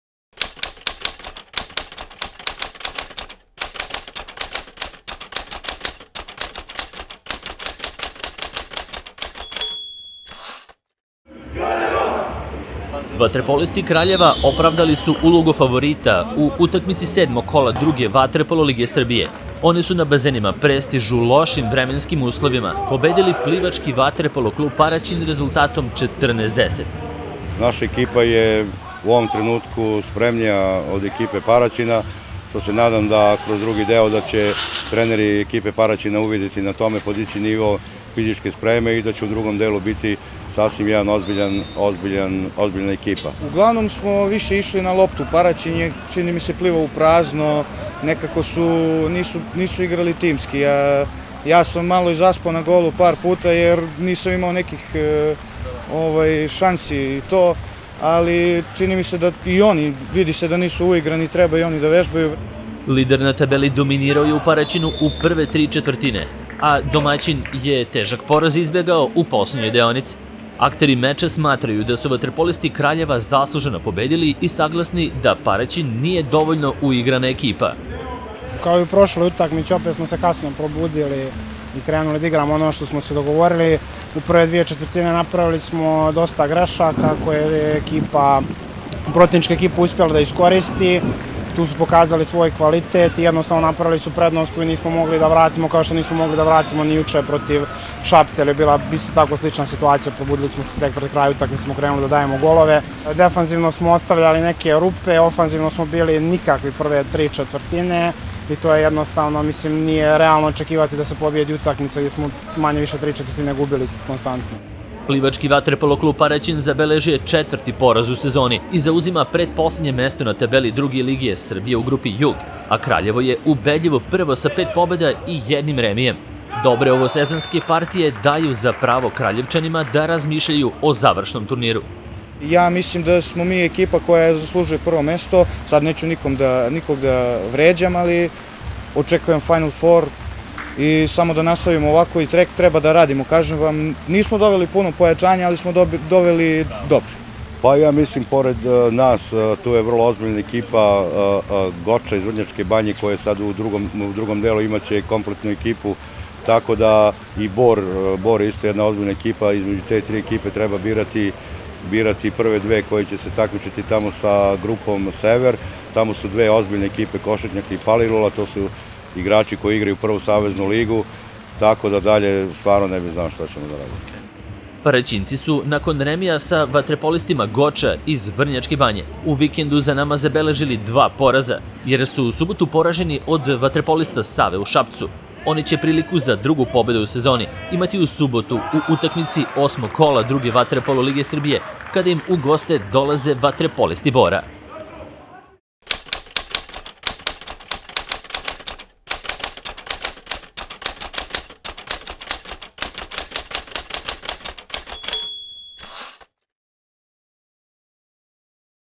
Vaterpolo, 2. liga Jug, PVK Paraćin - VK Kraljevo
Paraćinski vaterpolisti su u nedelju pretpeli četvrti ovosezonski poraz. Oni su nakon neuspeha u Šapcu od Save 12:9 juče poraženi od lidera na tabeli grupe Jug, Kraljeva rezultatom 14:10.Ovim porazom Paraćinci su najverovatnije ostali bez šanse za plasman na završni turnir a u subotu će na Bazenima Prestiž dočekati drugoplasirani Bor.